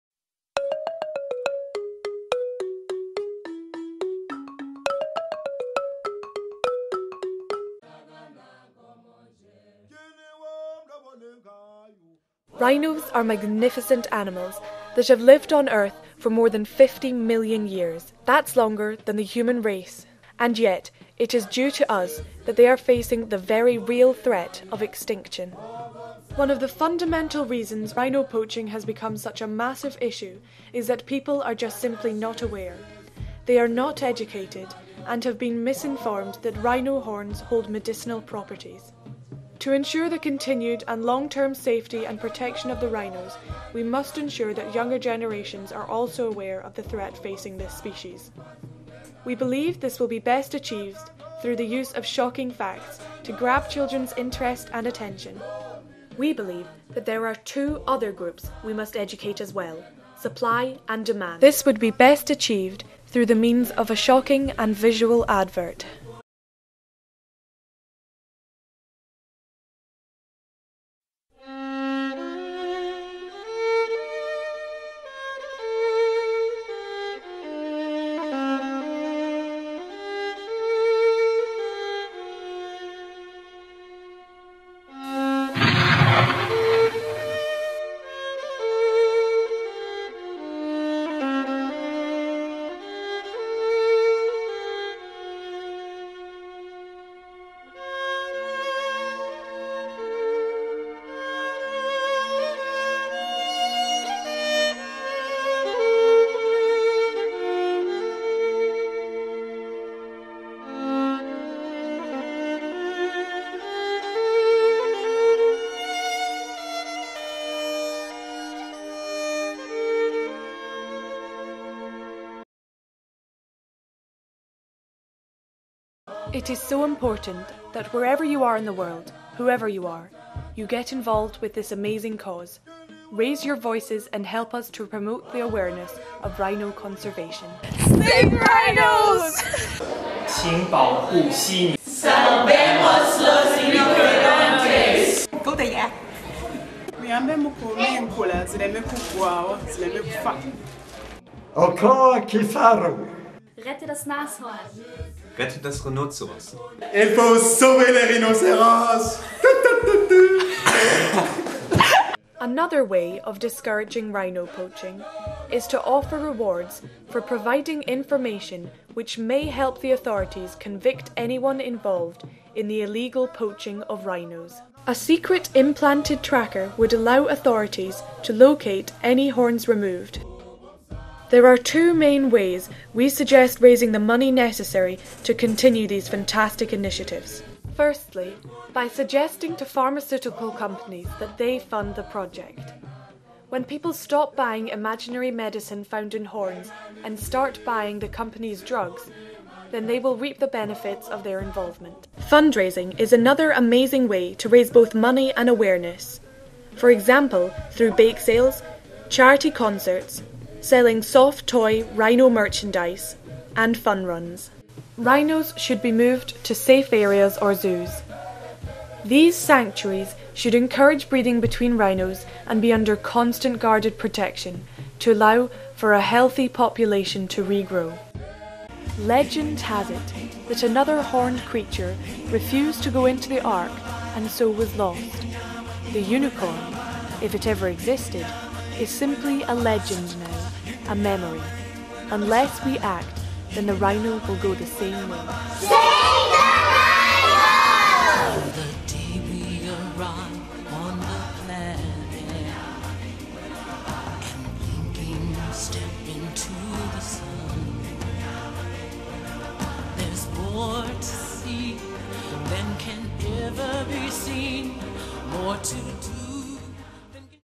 save the rhino project
Video for the Voices of Conservation competition to highlight the plight of the Rhino.